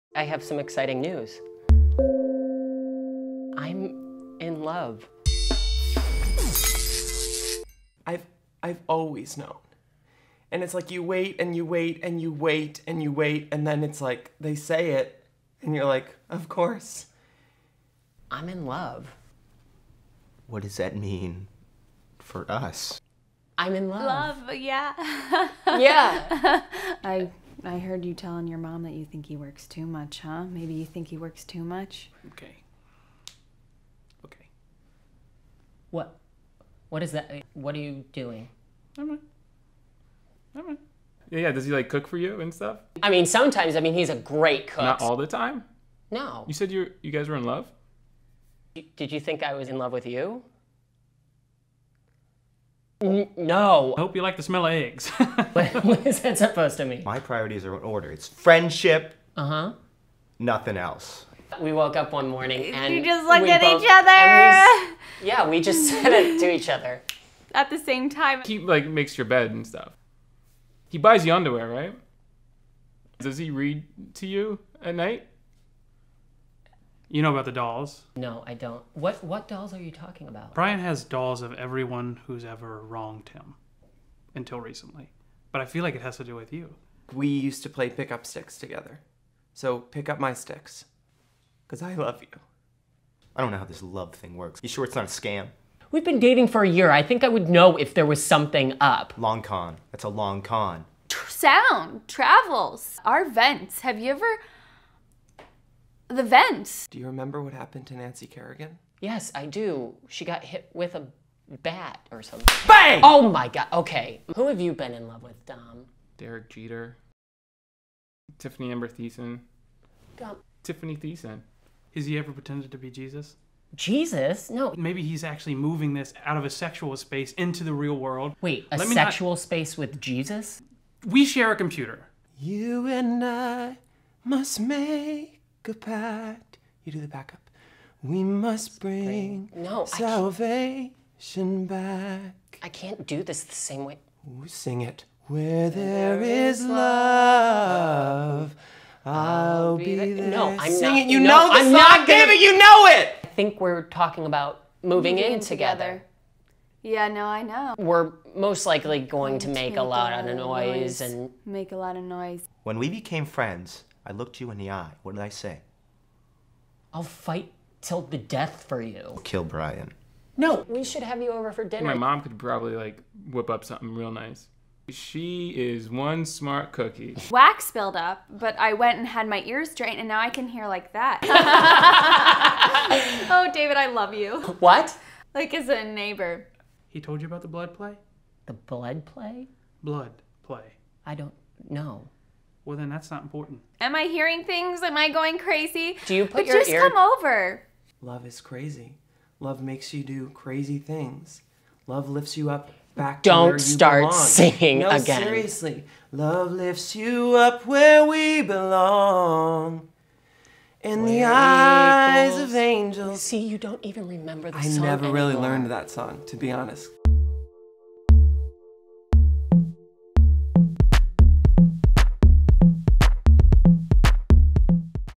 Part 2: WHAT TO EXPECT When You Tell Someone You're In Love
Coming out is hard. Especially when you know these people.

Conceived by David Craig

Starring: David Craig, Claire Ayoub, Erin Bartley, Caroline Cotter, Frank Hejl, Lydia Hensler, Mike Kelton, Dan LoPreto, Nick Guercio, Dom Manzolillo, & Keaton Patti.